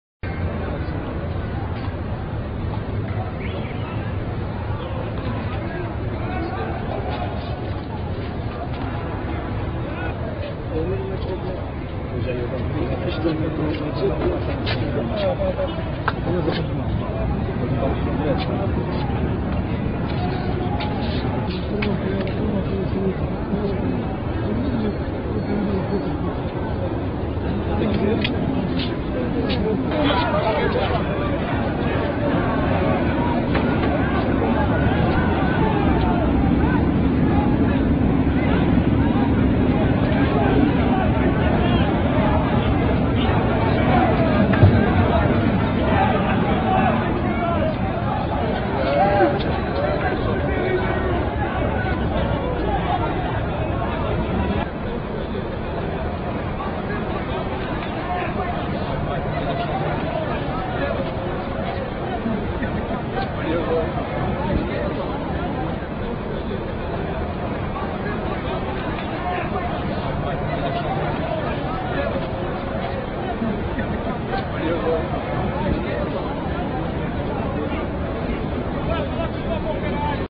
Confusão em Alvalade!!
Vários adeptos do Legia de Varsóvia causaram distúrbios junto ao estádio José Alvalade antes do encontro da 2ª mão da Liga Europa. Onze adeptos do clube polaco foram detidos e proibidos de ir ao jogo.

O momento de maior tensão, ainda assim, aconteceu pouco passava das 17 horas, quando os adeptos leoninos avistaram um grupo de polacos e avançaram contra eles. Mas umas bastonadas colocaram um ponto final naquela triste situação.
Pouco depois, com alguns petardos à mistura, os adeptos leoninos tentaram outra investida, mas dessa vez nem sequer conseguiram chegar perto dos adeptos do Legia, pois a PSP não permitiu. Pelo meio, um ferido aqui, outro ali, na maioria consequência das bastonadas dos agentes de autoridade.